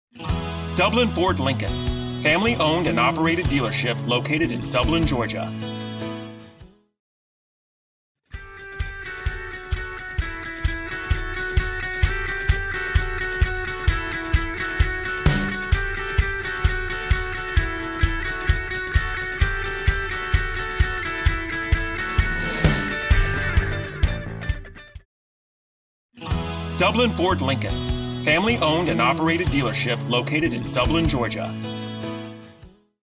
USED 2019 FORD MUSTANG EcoBoost Premium Fastback at Dublin Ford Lincoln (USED)
This is a USED 2019 FORD MUSTANG EcoBoost Premium Fastback offered in Dublin Georgia by Dublin Ford Lincoln (USED) located at 2265 Veterans BLVD, Dublin, Georgia

Stock Number: T2657A